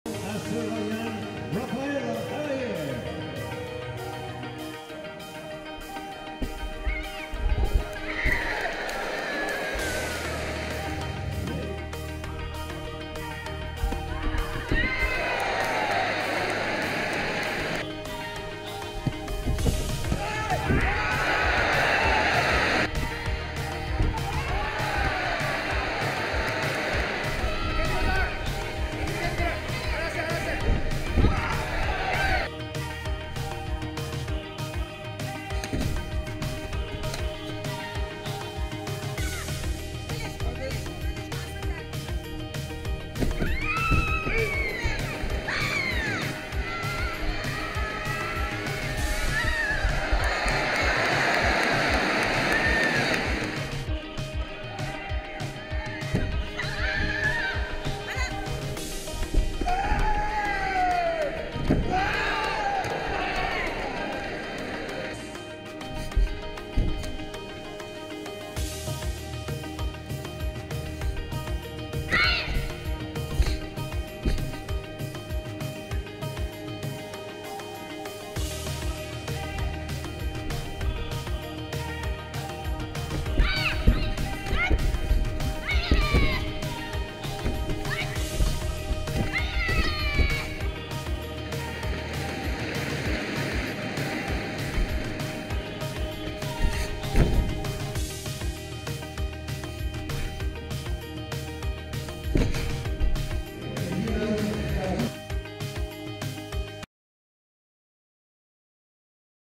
Thrilling KARATE coming to you!
The Karate 1-Premier League FUKUOKA is coming next week. To celebrate it, here we give you the best moments of the last Karate 1-Premier League in Japan!

Official YouTube channel of the World Karate Federation